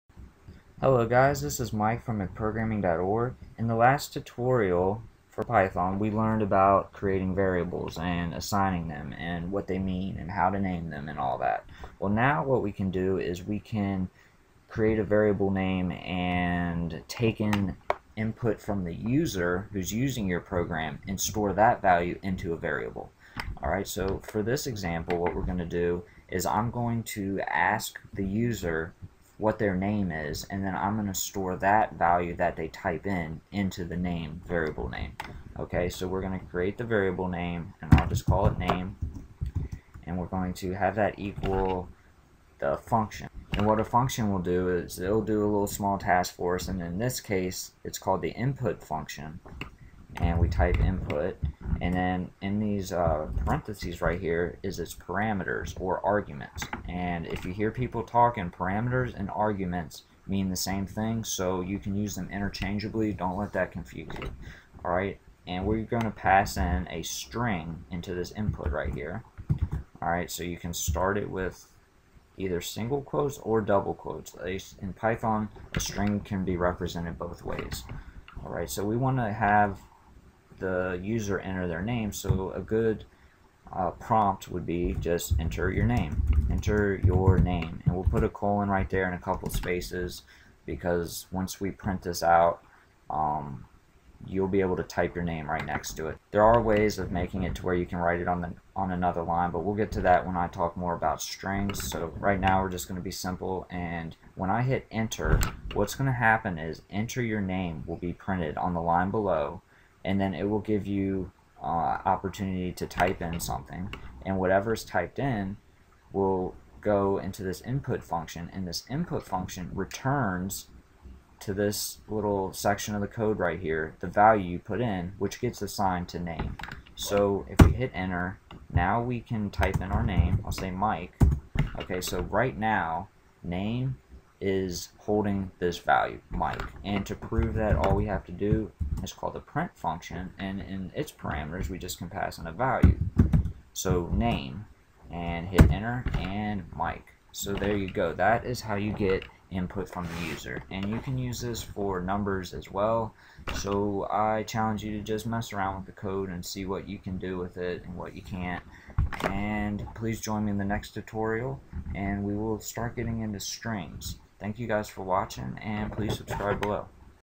Python - Getting Input from user (strings)
Python - Getting Input from user (strings) - In this python tutorial, I show you how to ask the user to enter some value which then will be stored in a variable in your program. Be sure to watch the next video on the playlist to learn how to take in numbers correctly.